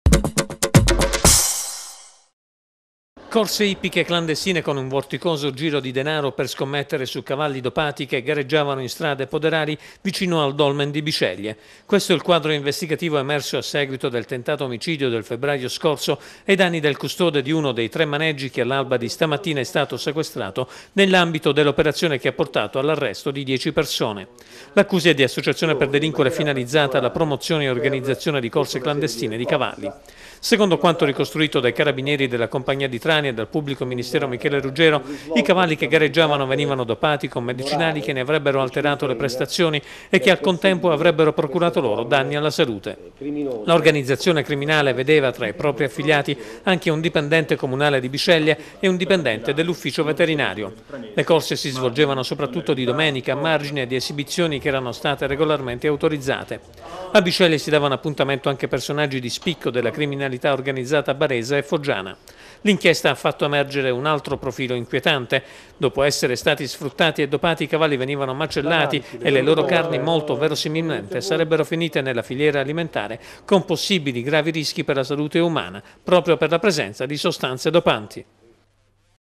TG 13.11.09 Corse e scommesse clandestine, 10 arresti in Puglia
I carabinieri hanno sgominato tra la Bat e la provincia di Foggia una organizzazione criminale che organizzava corse clandestine di cavalli. Gli animali venivano dopati e maltrattati e quando non erano più in grado di correre erano destinati alla macellazione.
Il servizio di Antonello Norscia.